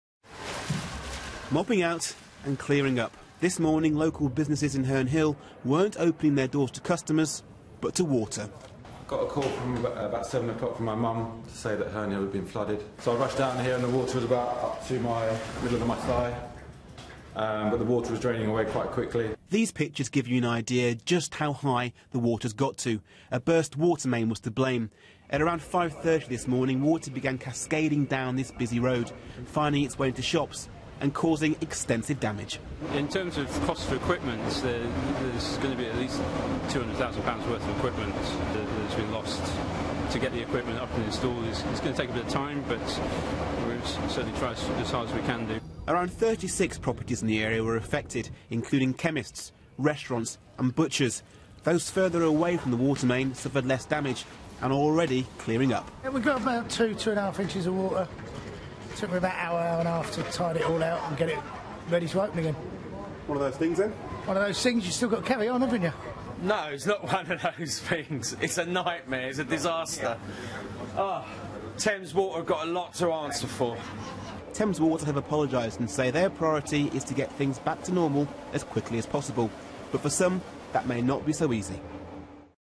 Burst water main floods London street
A clean-up operation is under way after a burst water main flooded homes and businesses in south London.

Fire crews had to use boats to navigate the flood water in Herne Hill.

London Fire Brigade said crews were working to pump water out of the flooded basements of 37 homes and businesses.

Emergency services have warned people to stay inside.